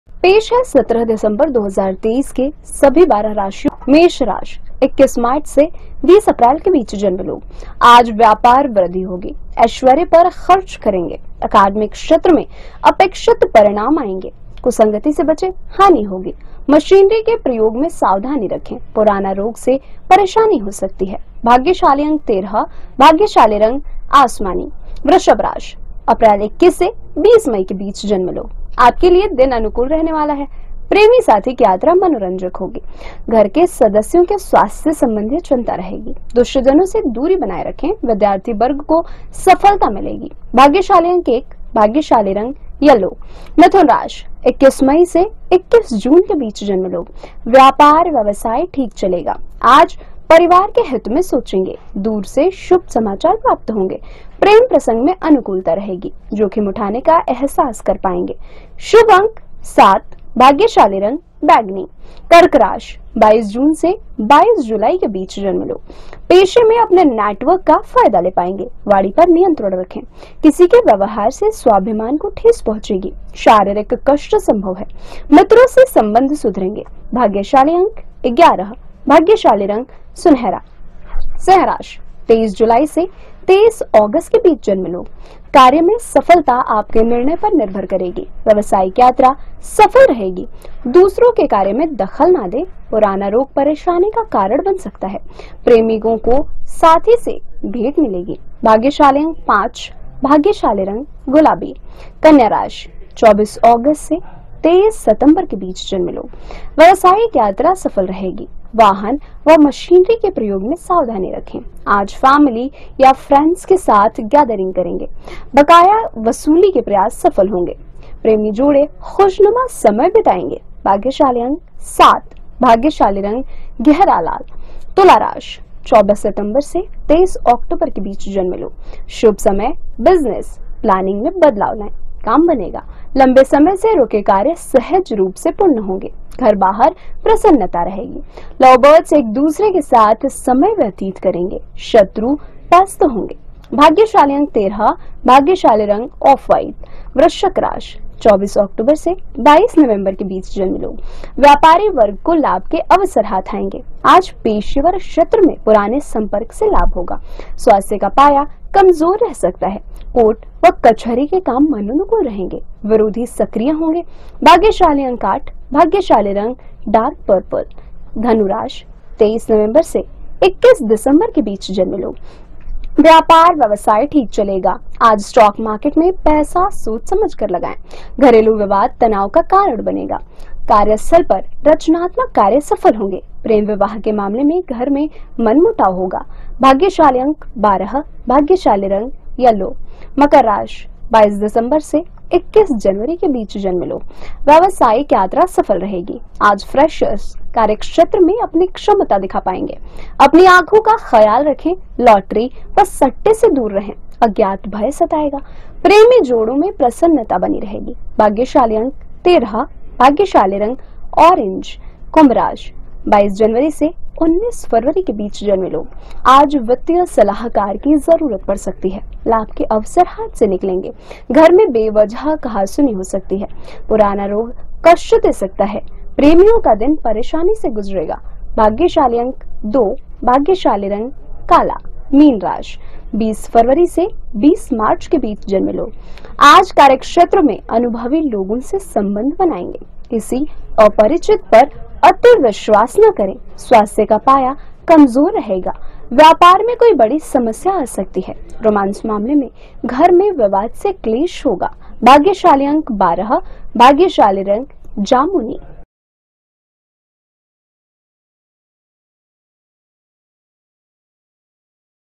Aaj Ka Rashifal 17 December: आज का दिन इन राशि वालों के लिए है खास। Astrology | 12 राशियों का राशिफल
पेश है 17 दिसंबर 2023 का सभी 12 राशियों का राशिफल।